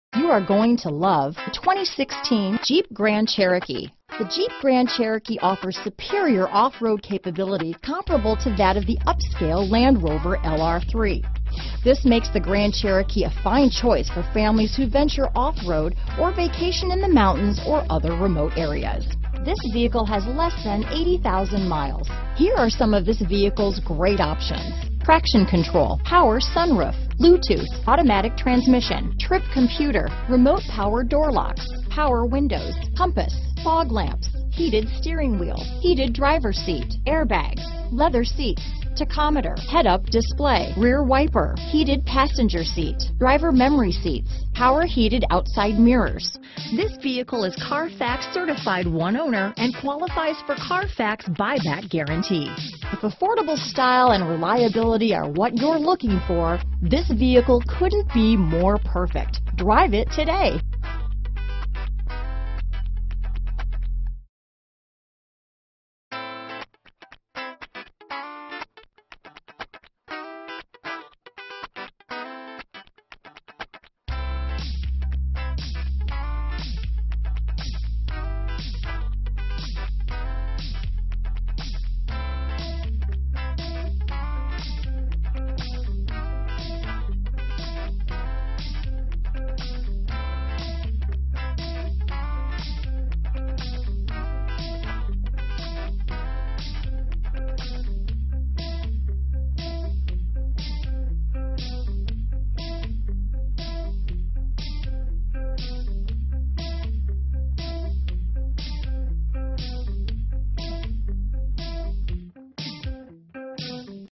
2016 Jeep Grand Cherokee Fall River, Dartmouth, New Bedford, Wareham, MA, Tiverton, RI 16727
True Blue Pearlcoat  2016 Jeep Grand Cherokee available in Fall River, Massachusetts at Prestige Auto Mart 1. Servicing the Dartmouth, New Bedford, Wareham, MA, Tiverton, RI area.
2016 Jeep Grand Cherokee Limited - Stock#: 16727 - VIN#: 1C4RJFBGXGC334384

4 Wheel Drive, never get stuck again** CARFAX 1 owner and buyback guarantee!!! Optional equipment includes: Radio: Uconnect 8.4 NAV, Power Sunroof...
Radio: Uconnect 8.4 NAV, Power Sunroof, 3.6 liter V6 DOHC engine, 4 Doors, 4-wheel ABS brakes, 8-way power adjustable drivers seat, Air conditioning with dual zone climate control, Audio controls on steering wheel, Automatic Transmission, Bluetooth, Clock - In-radio display, Compass, Driver memory seats, Engine hour meter, External temperature display, Four-wheel drive, Front fog/driving lights, Front seat type - Bucket, Head airbags - Curtain 1st and 2nd row, Heated drivers seat, Heated passenger seat, Heated steering wheel, Interior air filtration, Knee airbags - Driver, Leather seats, Memory settings for 2 drivers, Multi-function remote - Trunk/hatch/door/tailgateremote engine start, Passenger Airbag, Power Activated Trunk/Hatch - Power liftgate, Power heated mirrors, Power windows with 2 one-touch, Rear spoiler - Lip, Rear wiper, Remote power door locks, Roof rack - Rails only, Stability control - With Anti-Roll, Tachometer, Tilt and telescopic steering wheel, Traction control - ABS and driveline, Transmission hill holder, Trip computer, Universal remote transmitter